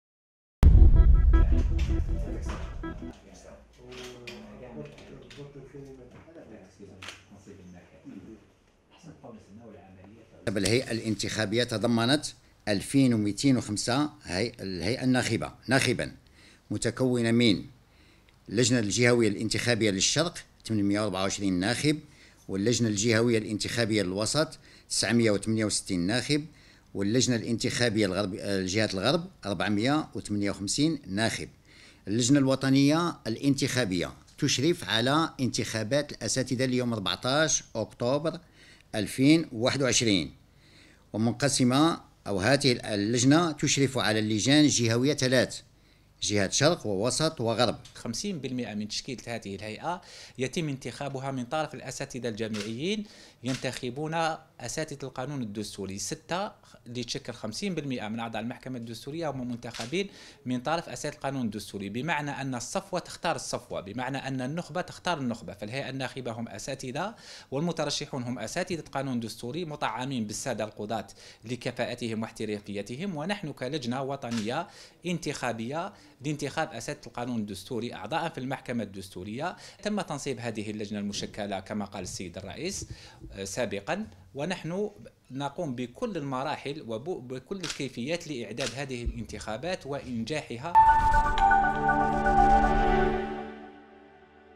اللجنة الوطنية لانتخاب أساتذة القانون الدستوري أعضاء المحكمة الدستورية تستكمل تحضيراتها
الجزائر (وأج) - استكملت اللجنة الوطنية لانتخاب أساتذة القانون الدستوري أعضاء المحكمة الدستورية تحضيراتها استعداد ليوم 14 أكتوبر 2021 .